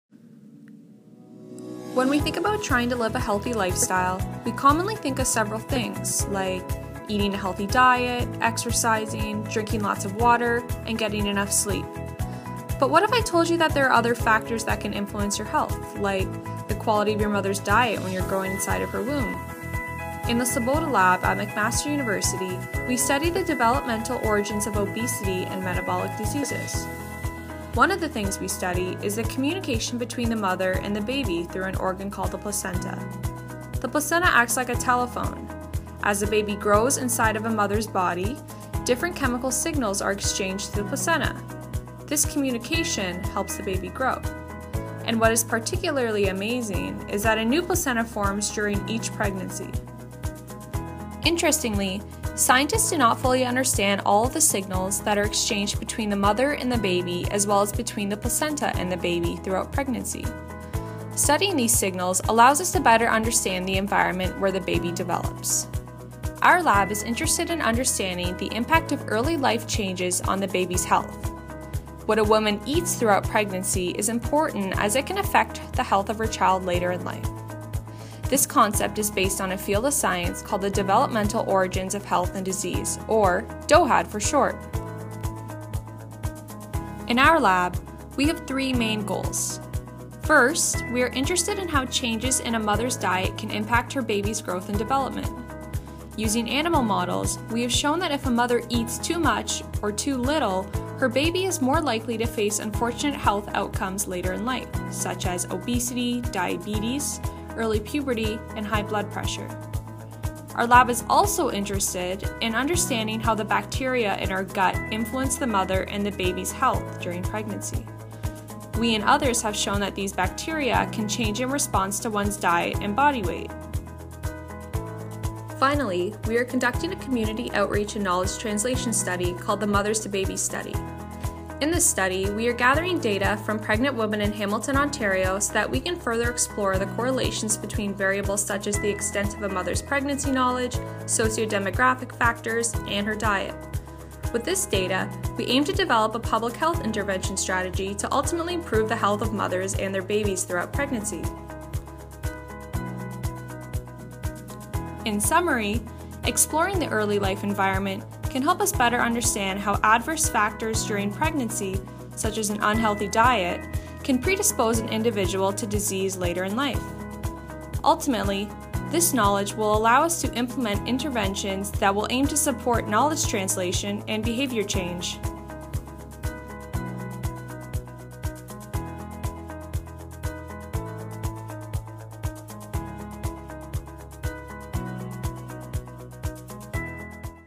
Investigating nutritional influences during early human development
Full title: Investigating how nutritional influences during early human development can affect the risk of developing chronic diseases later in life
Debbie (Yun Ya) Kao, McMaster University